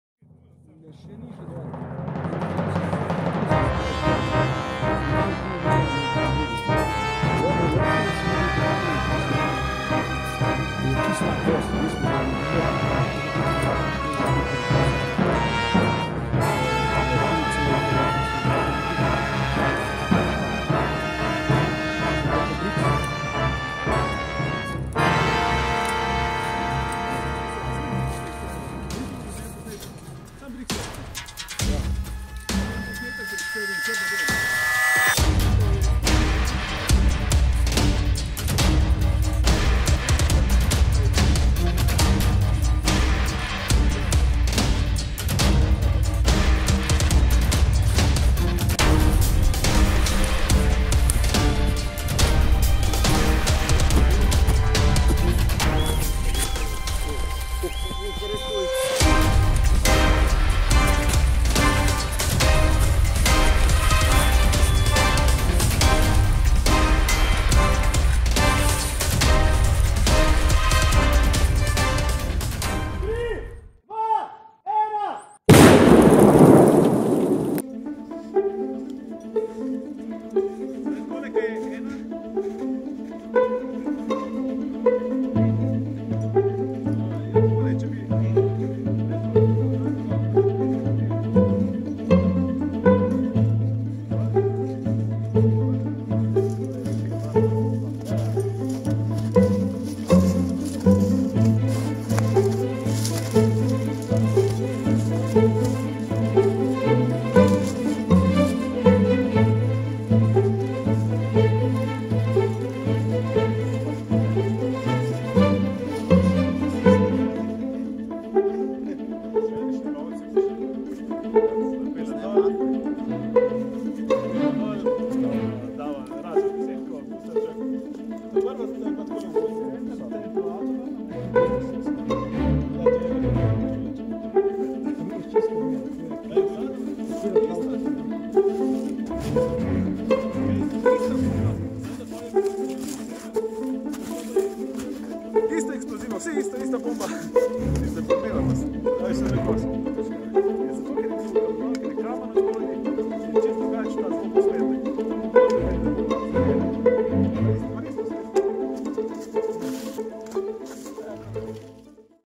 NUS Uničenje bombe Breda
Trije jamarji iz JK Borovnica smo v soboto 4. februarja v raziskovanju brezna nad vasjo Bukovje naleteli na italijansko bombo "Breda" iz druge svetovne vojne. Bombo smo pustili nedotaknjeno in se s tem izognili morebitnim resnim poškodbam če bi eksplodirala.

O najdbi smo obvestili center za obveščanje 112.
Včeraj, 12. februarja so bombo uničil pirotehniki.